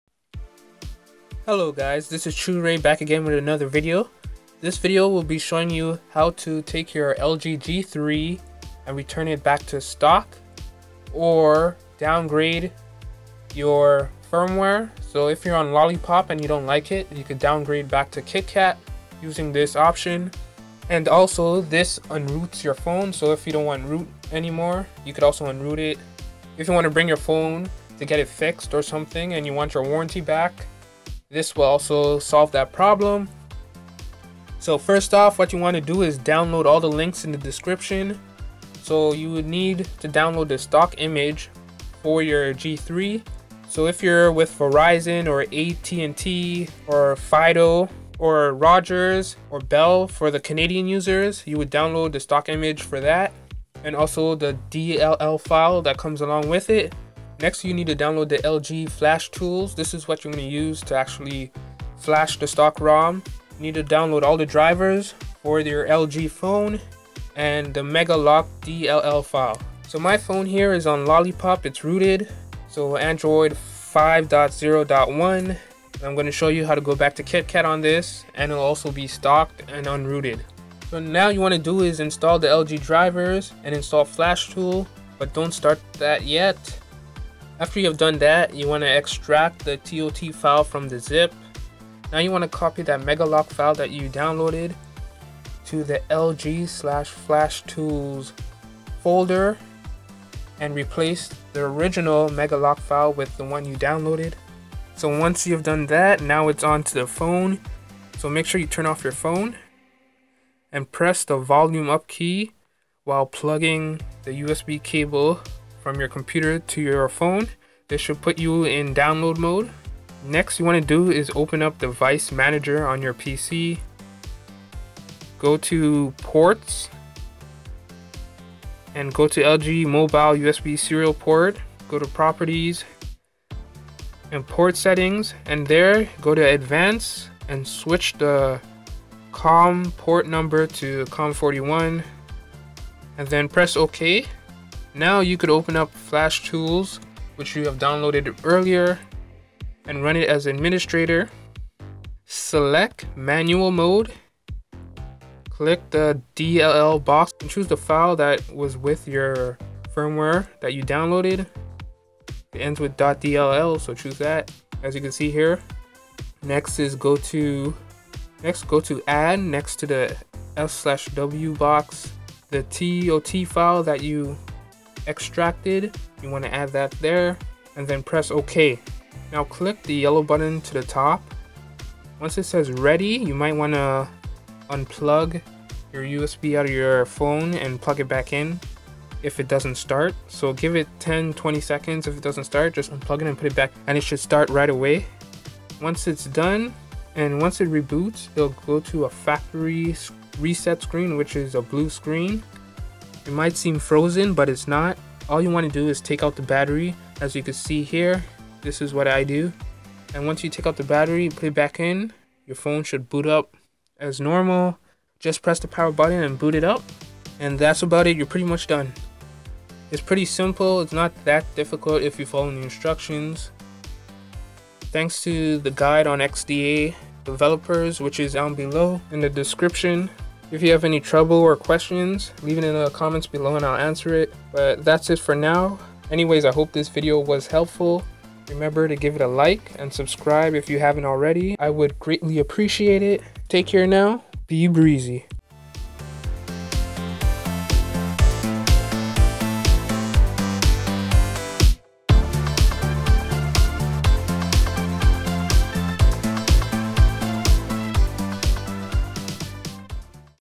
Return to Stock and/or Downgrade Firmware on LG G3
This is video on how you can return back to stock, downgrade and un-oot your LG G3 device.

Thanks to the guide on xda.

Warning: Make sure you fully understand the instructions before doing this. I am not responsible for any damage done to your device.